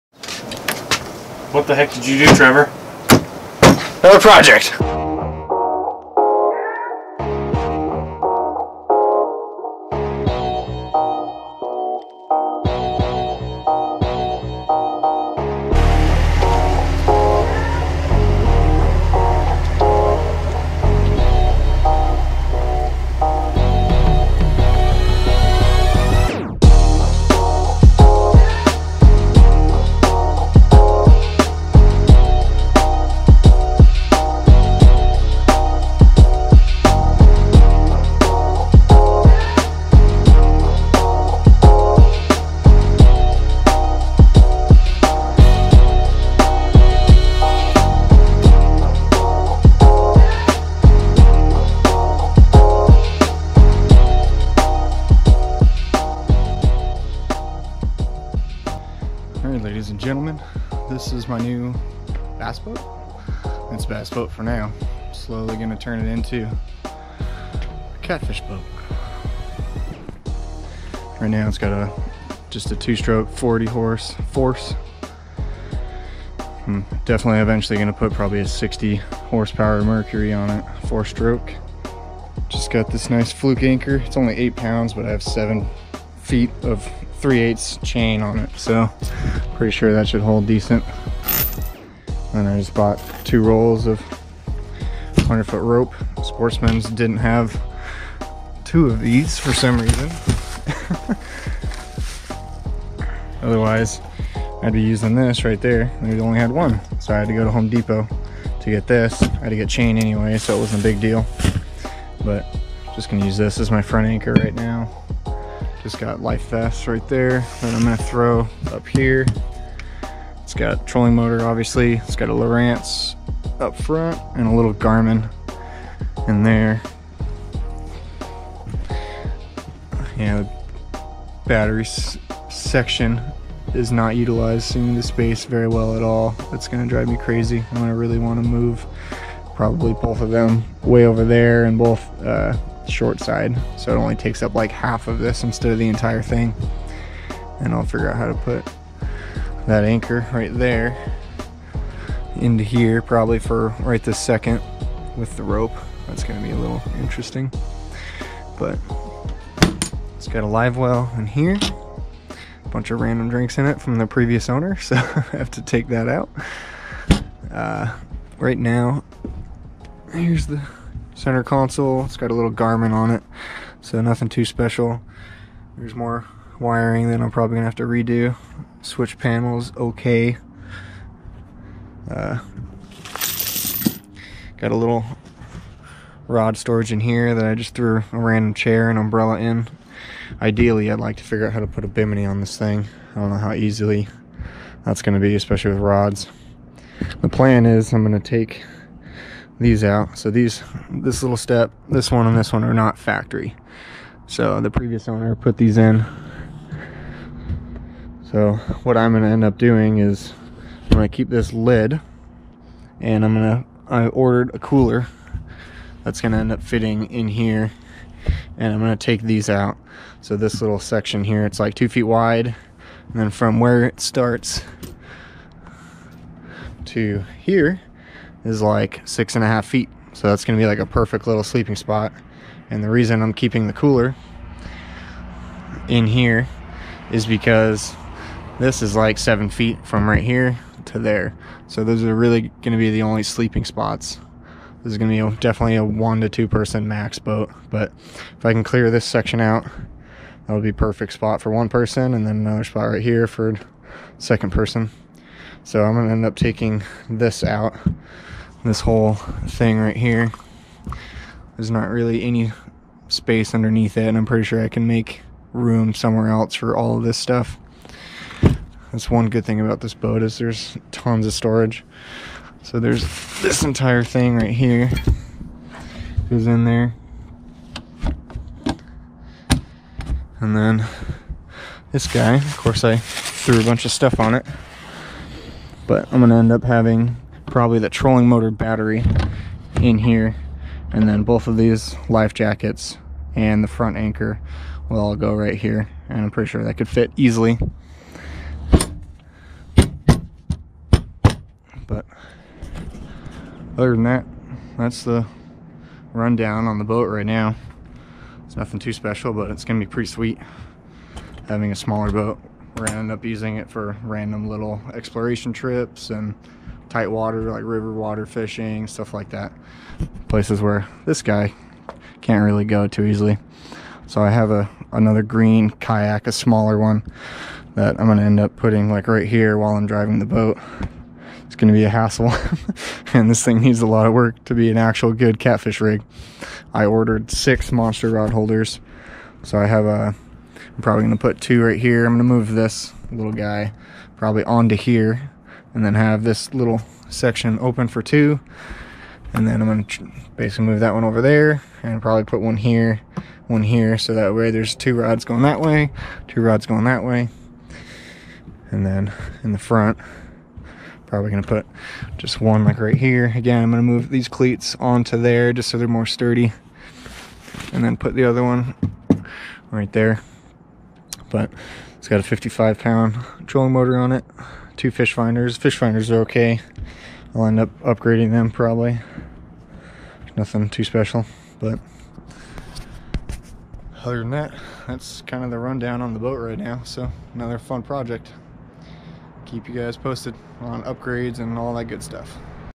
New Project! Turning a Bass Boat into a Catfish/Exploration Boat
New bass boat?? Just picked up this Spectrum Dominator 17. It’s going to be a blast working on it and getting it ready for over night trips!